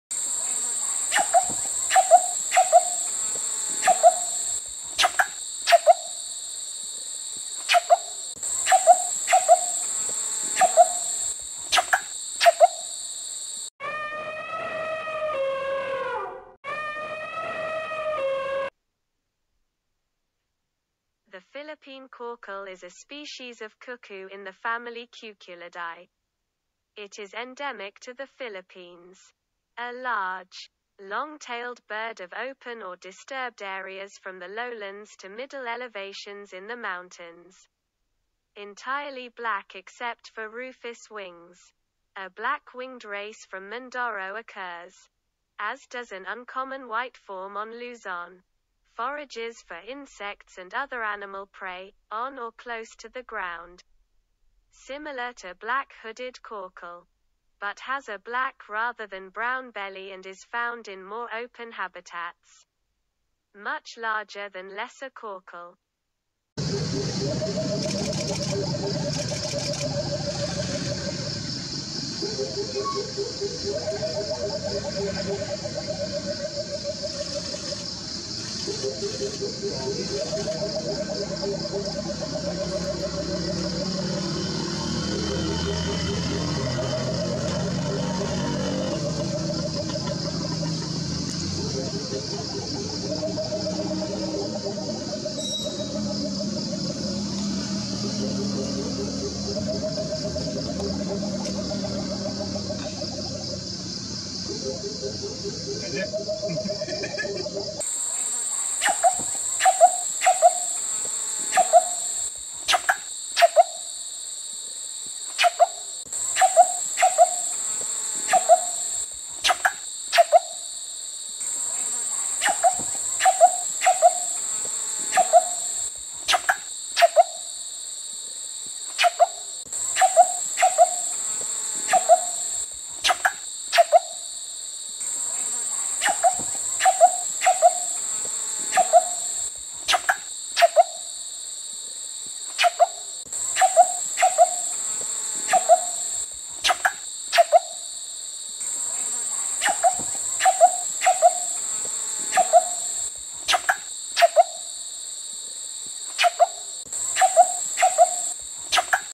Philippine coucal || Cuckoo chapter - 38 || The Rup animal
The Philippine coucal is a species of cuckoo in the family Cuculidae. It is endemic to the Philippines.
A large, long-tailed bird of open or disturbed areas from the lowlands to middle elevations in the mountains. Entirely black except for rufous wings. A black-winged race from Mindoro occurs, as does an uncommon white form on Luzon. Forages for insects and other animal prey, on or close to the ground. Similar to Black-hooded Coucal, but has a black rather than brown belly and is found in more open habitats. Much larger than Lesser Coucal.